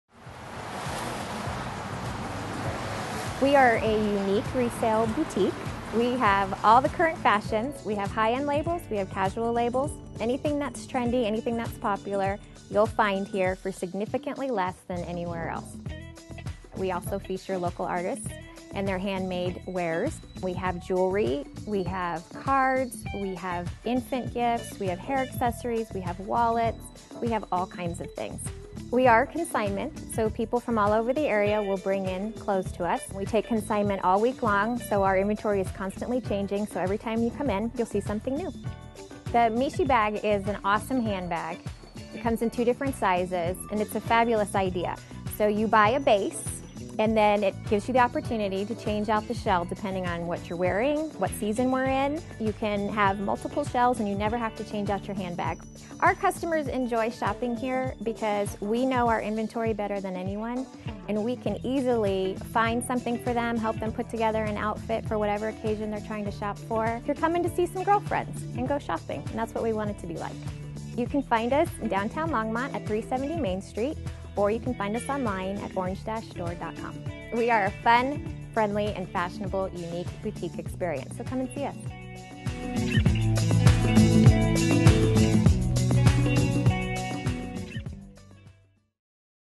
The Orange Door in downtown Longmont Colorado
An upscale consignment boutique in historical downtown Longmont, CO specializing in misses and juniors fashions, footwear, and accessories. We also feature one of the region's largest assortment of hand-made wearable art and jewelry.